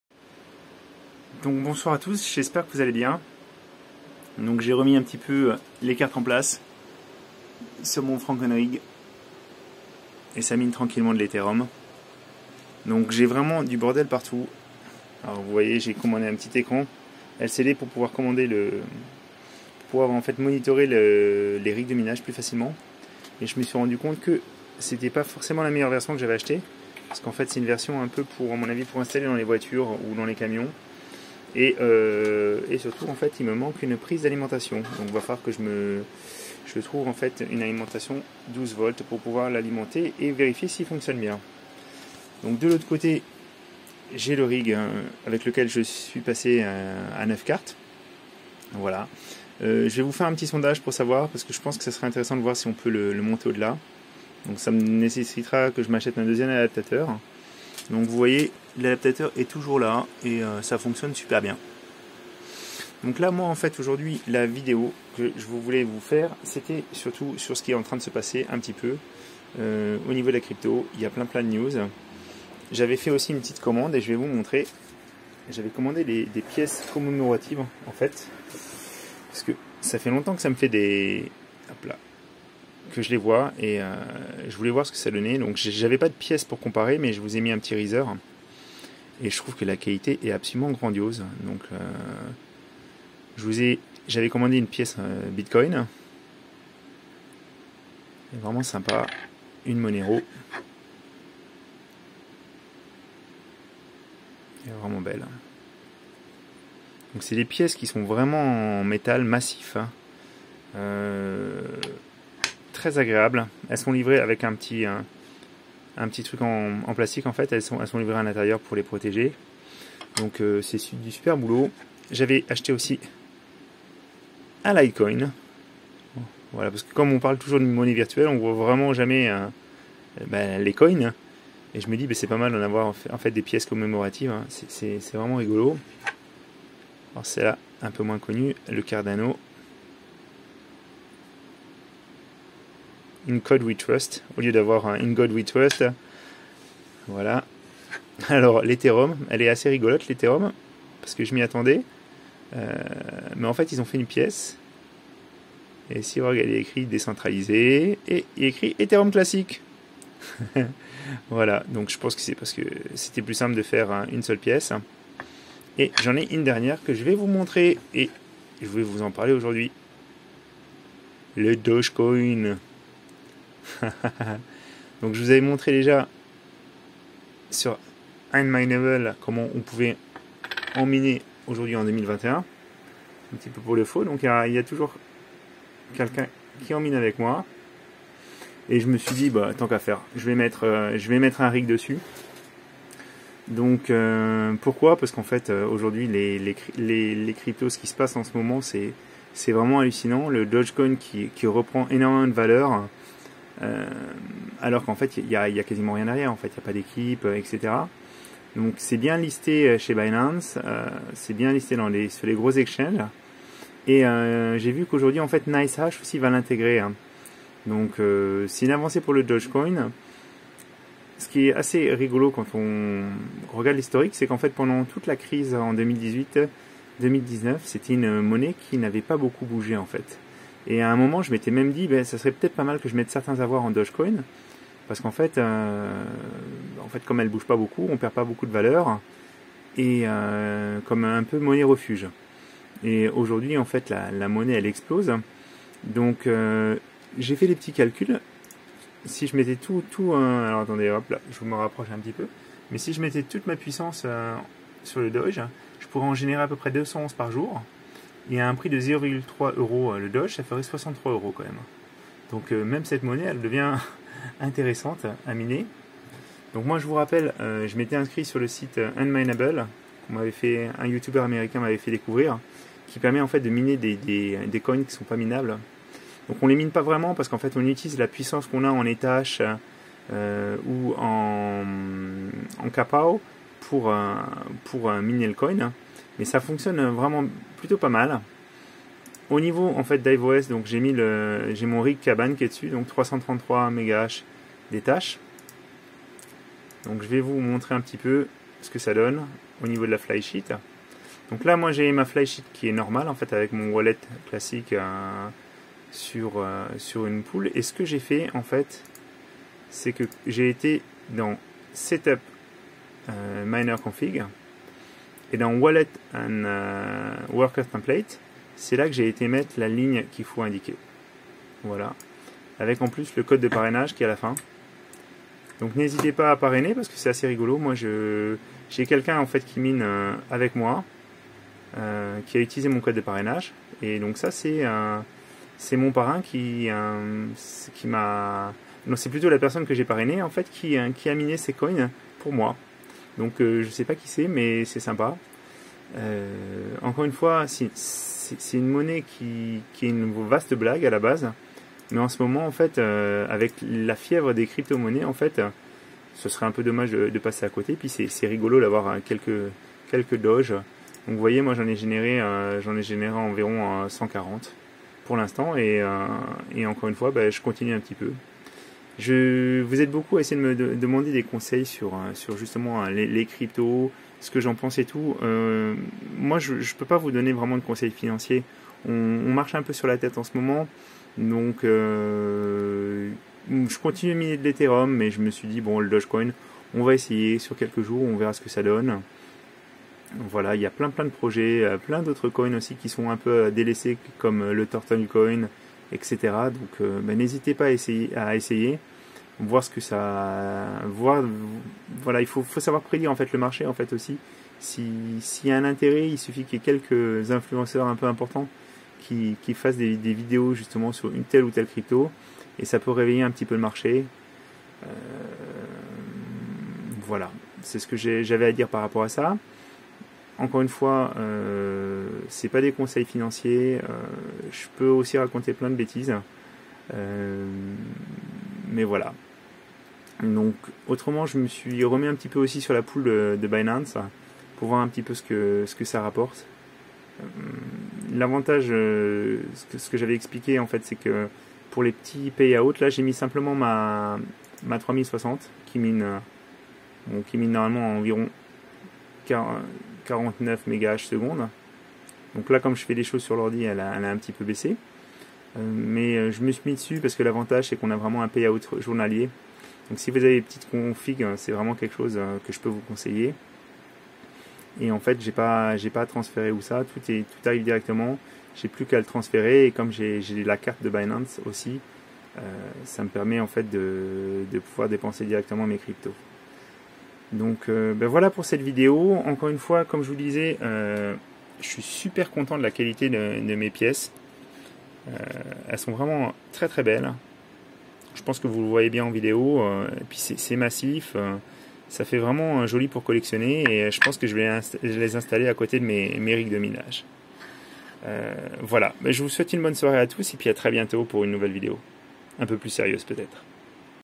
Mais que ce passe t'il avec le Dogecoin ?
Bonjour,

Dans cette vidéo je vous parle et je vous montre surtout  mes pièces commémoratives Cryptos

Je vous explique comment configurer votre feuille de route sous HiveOs pour miner du Dogecoin et vous rappel que si vous avez un faible Hashrate vous pouvez allez sur la Pool de Binance qui propose un payout journalier.

Mon lien pour le DOGE chez Unminable : qsez-z13f